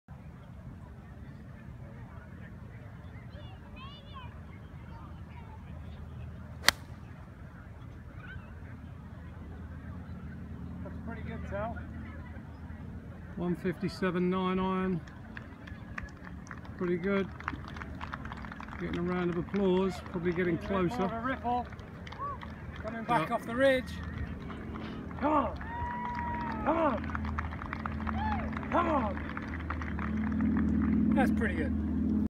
Ian Poulter taking a practice shot at Augusta
Ian Poulter is preparing for the Masters tournament at Augusta. Check out the video of him taking a practice shot in front of the crowd.

For more exclusive behind the scenes content from Ian Poulter at the Masters please sign up to our Clubhouse.